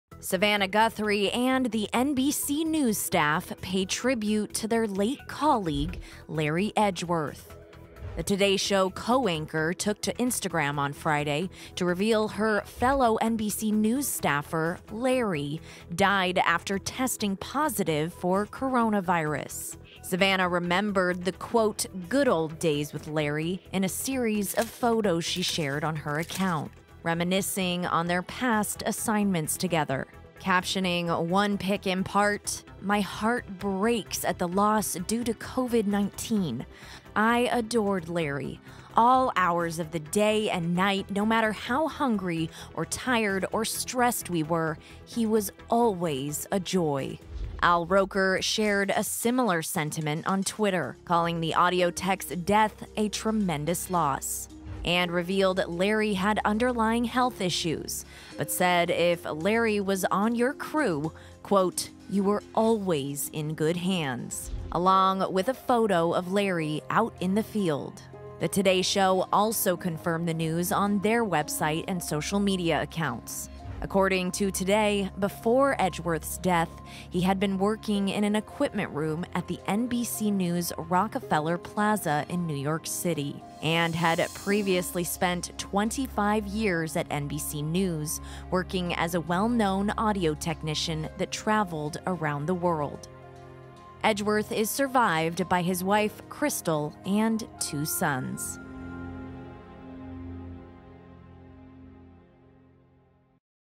Savannah Guthrie HEARTBROKEN Over Death of NBC Colleague
Savannah Guthrie revealed on Friday that her NBC News co-worker, Larry Edgeworth, died after testing positive for COVID-19.